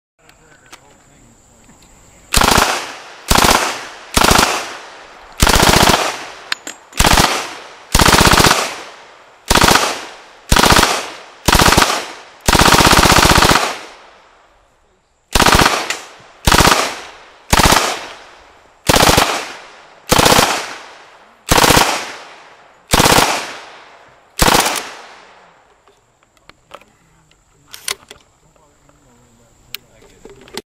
100 Round Colt Thompson Drum
Firing a "C" drum through a Colt Thompson Submachine Gun at the 2009 American Thompson Association Show and Shoot.